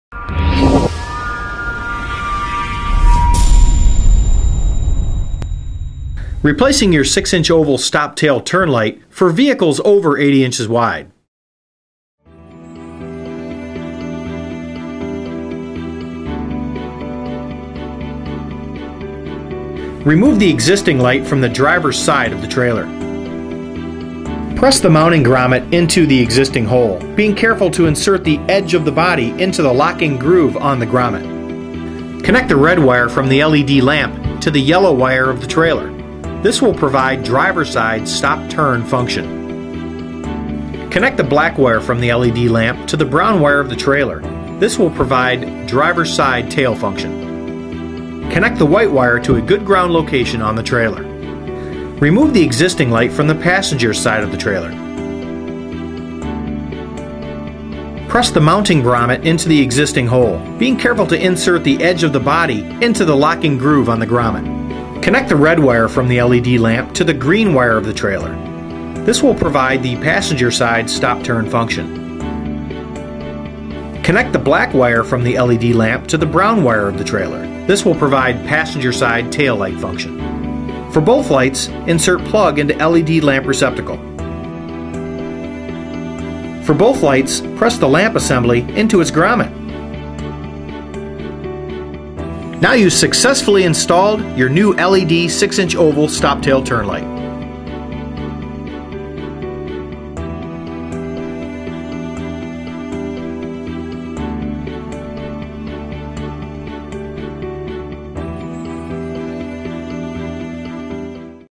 Blazer LED 6in. Oval Tail/Turn and Park Light Amber Model# C561ATM
Blazer LED amber 6in. oval tail/turn/park light is designed to conform to applicable US FMVSS108 and Canadian CMVSS safety standards. It contains high-output LEDs for exceptional brightness and pe...